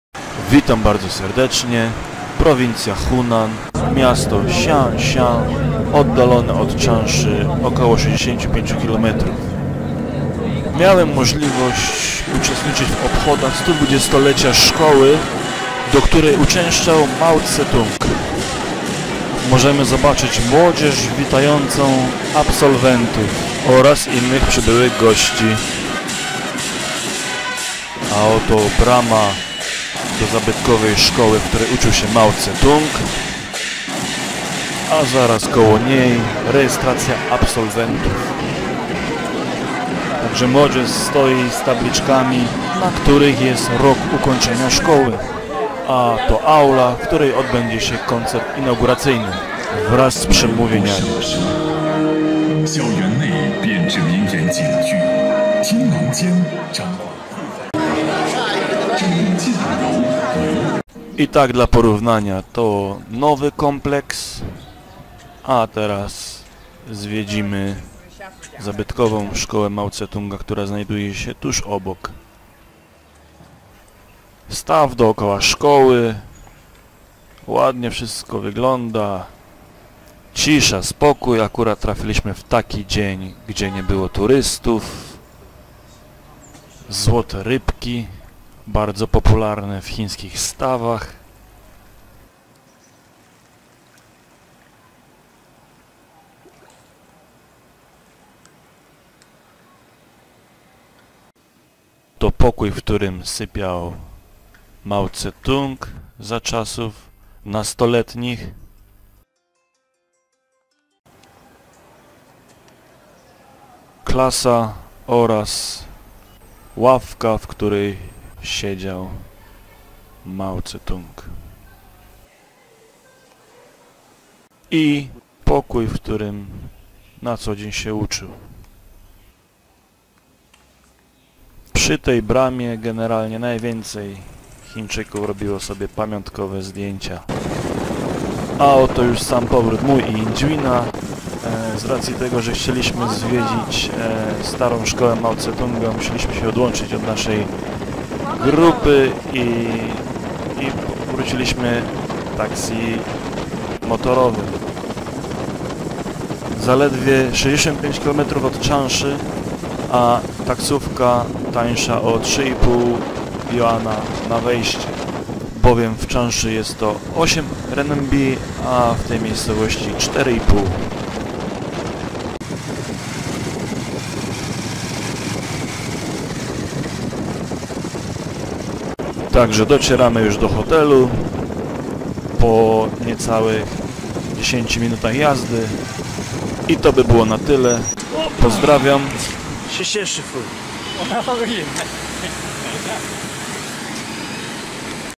#21 Odkrywanie Chin - Jubileusz 120-lecia szkoły Mao Zedong'a
Jubileusz 120-lecia szkoły, do której uczęszczał Mao Zedong.